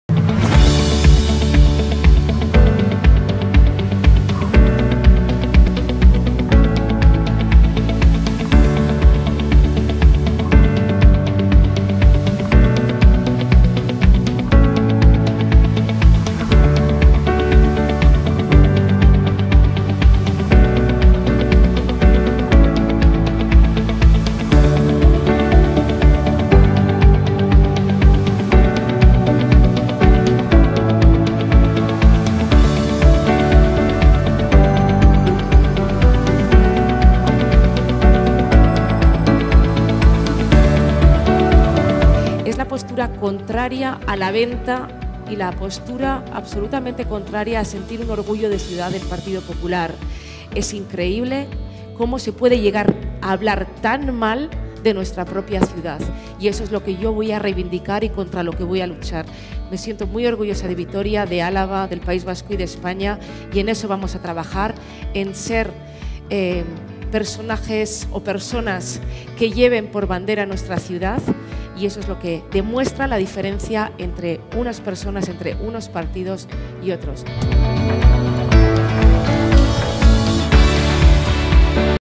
Apuesta socialista [cogobernanza e innovación] para un desarrollo sostenible del Turismo en Euskadi
Turismo en Euskadi

Maider Etxebarria: “Es increíble que el PP hable tan mal de Vitoria-Gasteiz. Yo voy a luchar contra eso. Me siento muy orgullosa de nuestra ciudad, de Álava, de Euskadi y de España”

Cristina González: “No sé si hay turismo de izquierdas y turismo de derechas, pero sí sé que hay turismo inclusivo y turismo excluyente. El turismo debe ser un signo de igualdad y progreso”

Javier Hurtado: “Sé que está de moda reivindicar, criticar o hacer notas de prensa muy duras para sacar cabeza o rédito político. Nosotros no trabajamos así. Trabajamos con nuestro teléfono, con nuestras reuniones, con la colaboración institucional en todos los niveles”

Fernando Valdés: “Hemos escuchado a Feijóo defender el plan de estabilidad; eso no son fondos europeos, son recortes”

La colaboración entre instituciones, la cogobernanza, contribuye de forma decisiva en la consecución de objetivos que tiene planteados el sector turístico en materia de sostenibilidad, una clave fundamental para su crecimiento y desarrollo ordenado. La financiación, con la ayuda de los fondos europeos, de los planes de sostenibilidad o el desarrollo de los llamados destinos turísticos inteligentes son algunos ejemplos.

Ésta es una de las principales conclusiones de la jornada “Turismo, transformación y desarrollo” que, organizada por el PSE-EE, ha reunido en Vitoria-Gasteiz a los cuatro niveles de la administración con responsabilidad en la gestión pública: el Ayuntamiento de la capital alavesa, la Diputación de Álava, el Gobierno Vasco y el Gobierno de España, todos ellos con dirección y liderazgo socialista.

La teniente de alcalde y concejala de Promoción Económica, Empleo, Comercio y Turismo, Maider Etxebarria, ha sido la conductora del encuentro, que también ha contado con la participación de la teniente de diputado y diputada foral de Fomento del Empleo, Comercio y Turismo, Cristina González; el consejero vasco de Turismo, Comercio y Consumo, Javier Hurtado; y el Secretario de Estado de Turismo, Fernando Valdés.

Para Maider Etxebarria, el Turismo constituye desde ya una actividad económica y social que los socialistas quieren poner sobre la mesa para explorar todas sus posibilidades cuando toca hablar del modelo de crecimiento que se desea para Vitoria-Gasteiz y para el conjunto del territorio alavés. La responsable municipal de esta área y de otras relacionadas con la economía y el empleo ha explicado que la digitalización del sector cobra cada vez más peso cuando se plantean posibilidades de mejora porque el viajero está “hiperconectado” y fomentar experiencias digitales o tecnológicas amplía las oportunidades para las empresas e instituciones.

“Las administraciones y las empresas podemos crear servicios relacionados con el turismo inteligente mezclando sostenibilidad e innovación”, ha explicado, tras plantear que el sector se encuentra inmerso en la ciudad en un proceso de transformación en el que intervienen factores como la economía mundial, la pandemia, la conciencia medioambiental o el avance tecnológico.

La teniente de alcalde ha detallado las características del proyecto Destinos Turísticos Inteligentes (DTI), del que forma parte la capital alavesa, una iniciativa de la Sociedad Mercantil Estatal para la Gestión de la Innovación y las Tecnologías Turísticas, Segittur, impulsada por la Secretaría de Estado de Turismo.